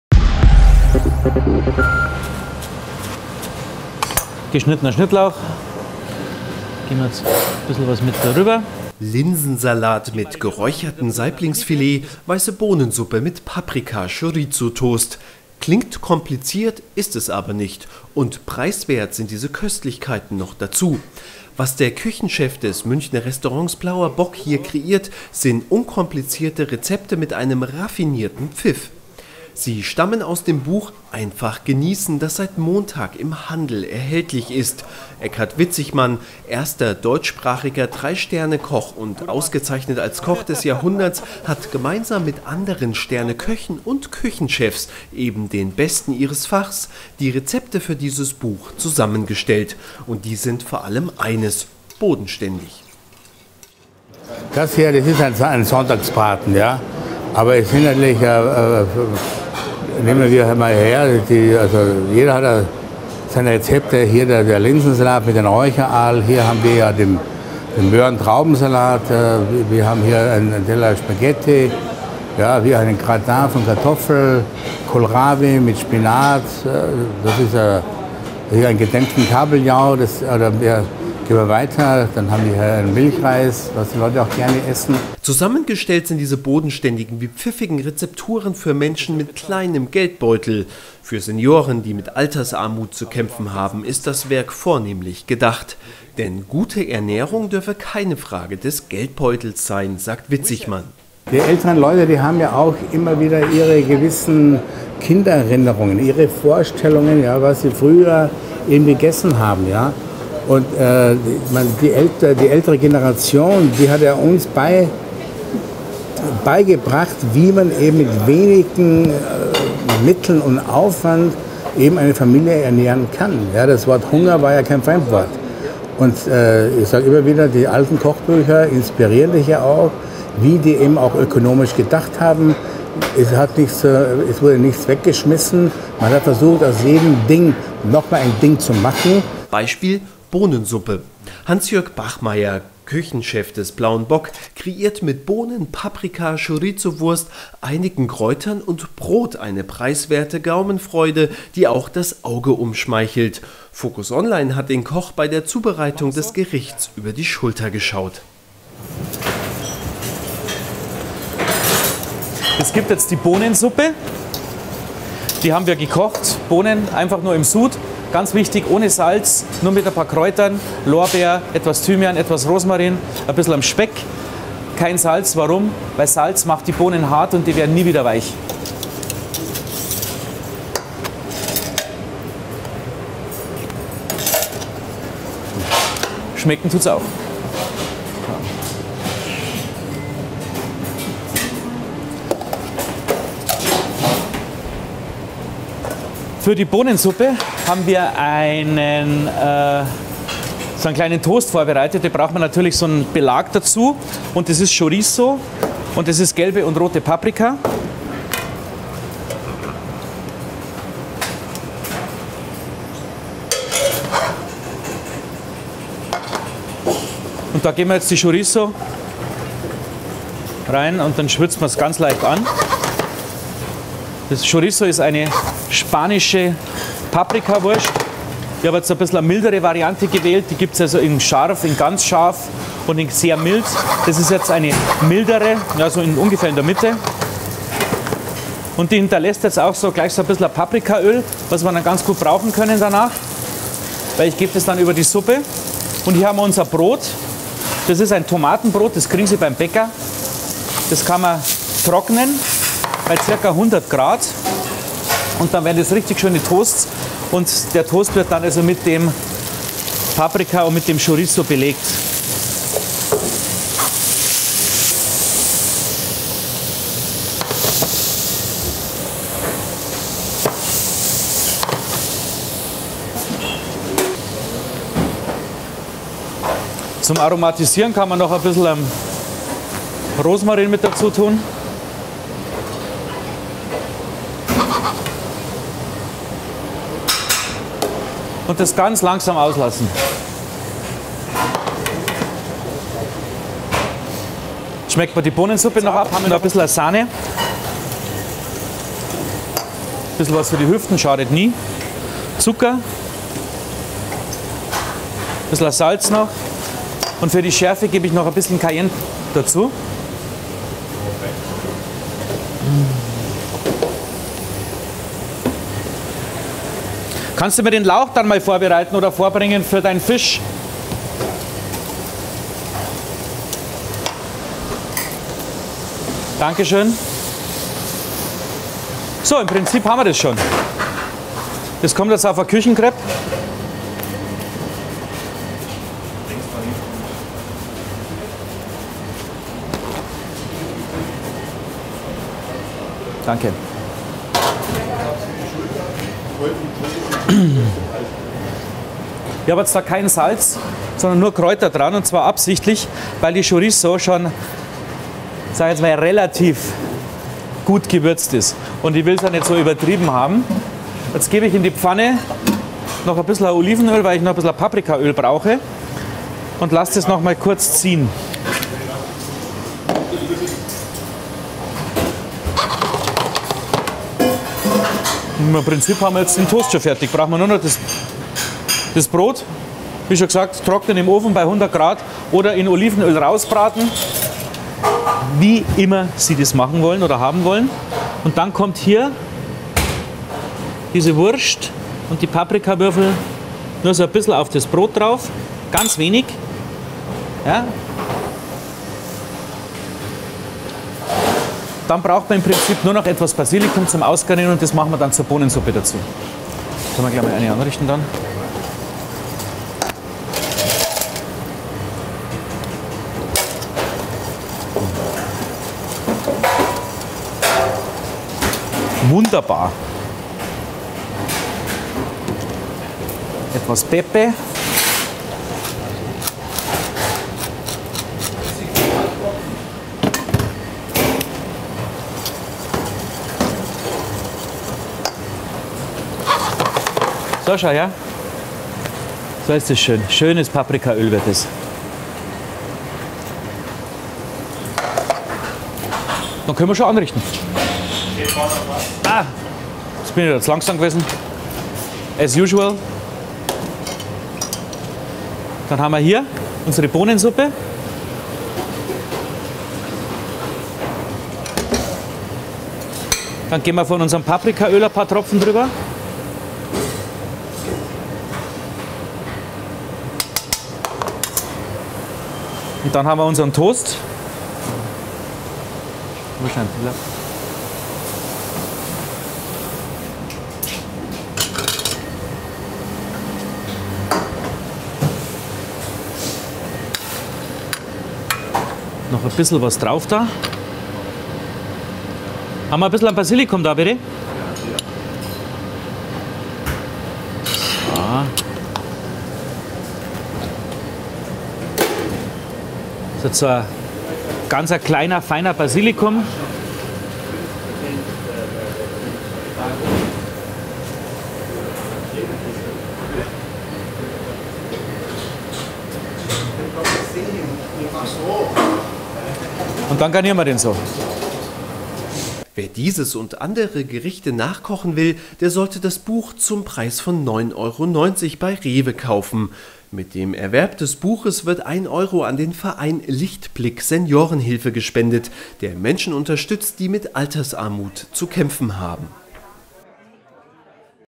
Sterne-Köche verraten simple Tricks für raffinierte Gerichte
- Unkomplizierte wie preiswerte Gerichte, die dennoch mit einem raffinierten Pfiff den Gaumen überraschen, haben jetzt der Drei-Sterne-Koch Eckart Witzigmann und seine hochdekorierten Kollegen in einem Buch vorgestellt. FOCUS Online war beim Probekochen dabei.